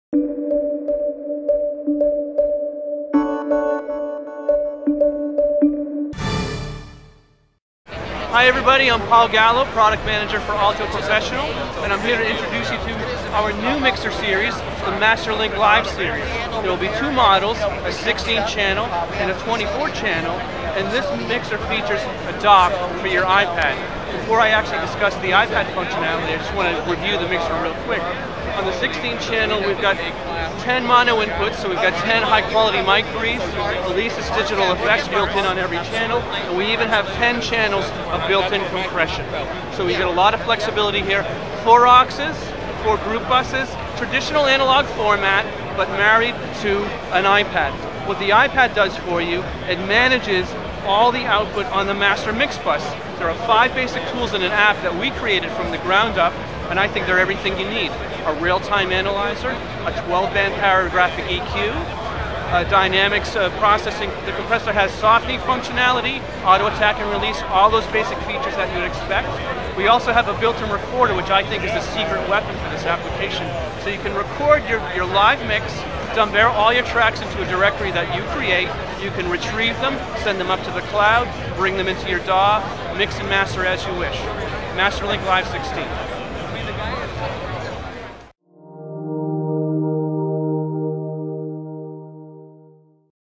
Alto Masterlink Live 16
This 16-channel mixer uses an iPad for various functions, including real-time analysis, equalization, and recording.